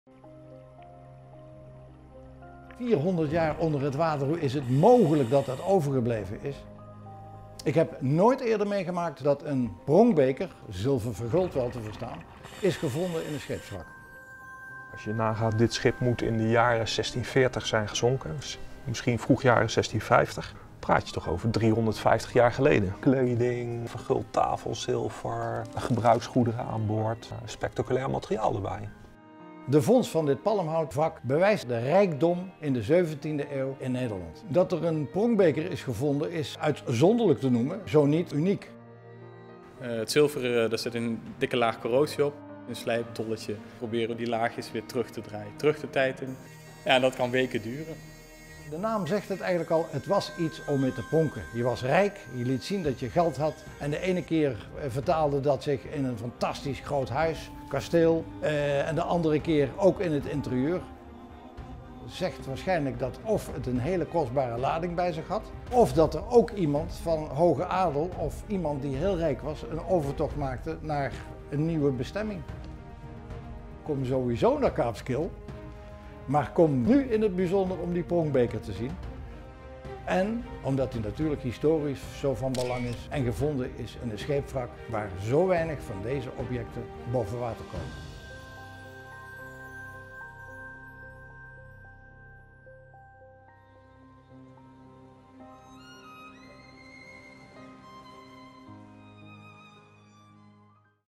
Pronkbeker uit het Palmhoutwrak I Museum Kaap Skil
“Een schip dat spullen vervoerde voor heel rijke, mogelijk zelfs koninklijke personen, lag 400 jaar begraven onder het zand op de zeebodem.”

Een aantal jaren geleden werd een unieke collectie archeologische objecten in de Waddenzee ontdekt. In het Palmhoutwrak, een wrak van een groot houten zeilschip, waren persoonlijke bezittingen uit de rijkste jaren van de Gouden Eeuw gevonden. Naast de onderdelen van een garderobe van adellijke herkomst, waaronder een bijna gave zijden japon, bevatte de vondst ook zilverwerk. Een verguld zilveren pronkbeker uit deze collectie is nu minutieus gerestaureerd en vanaf 9 maart te zien in de tentoonstelling ' Pronkstuk uit het Palmhoutwrak'.